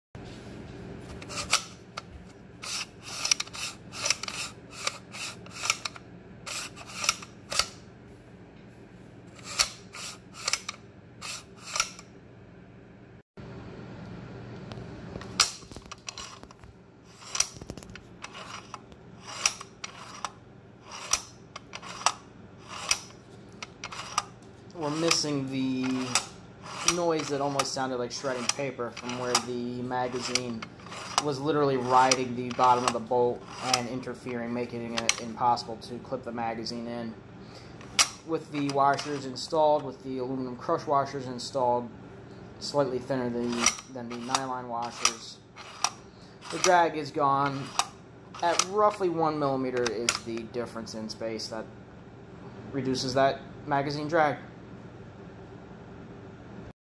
Shimming A Magpul Magwell To Eliminate Magazine To Bolt Interference
Not the best video lol, but if you're installing Magpul's Hunter Stock & magwell & getting interference, shimming the magwell down approximately 1mm, by using 1mm thick crush washers between the stock & magazine well, will eliminate the interference. There were no issues cycling through 50 rounds after the magwell was shimmed, every round loaded successfully & the magazine was able to click into place with normal effort.